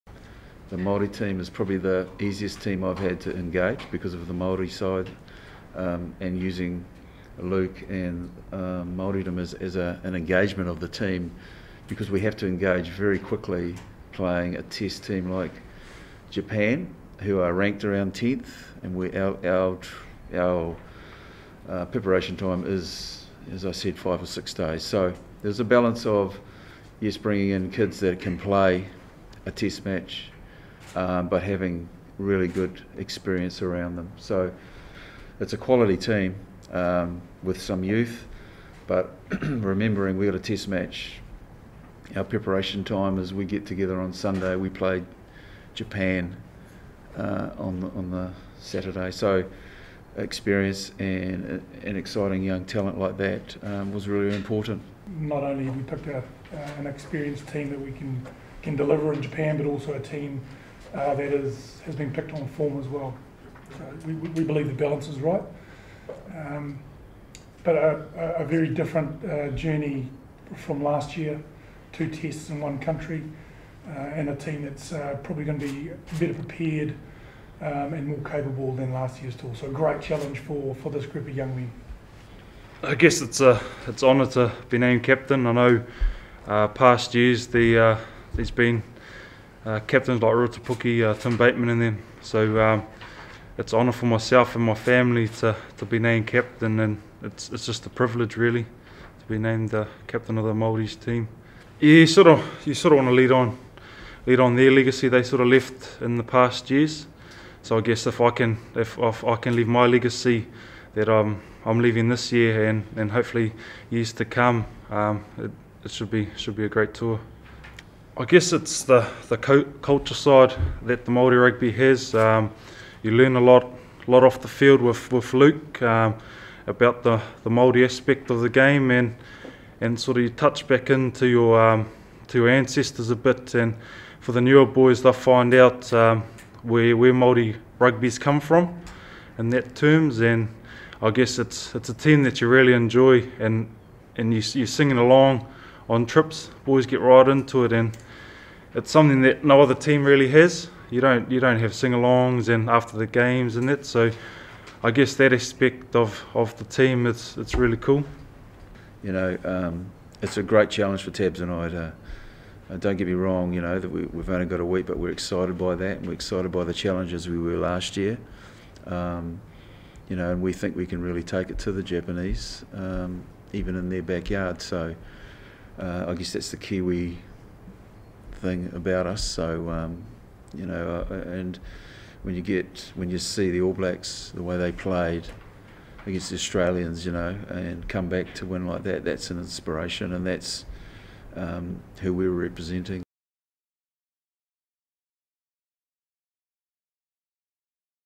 Maori All Blacks coaches talk about the tour to Japan
Hear from Head Coach Colin Cooper, Assistant Coach Tabai Matson and Captain Charlie Ngatai as they get ready to take on Japan in two matches.